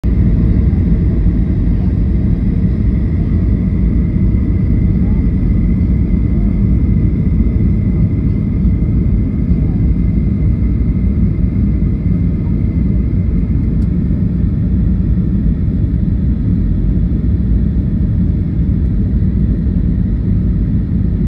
Landing at Victoria airport (2)
This is part II of our landing at the airport in Victoria, British Columbia, Canada in the summer of 2022.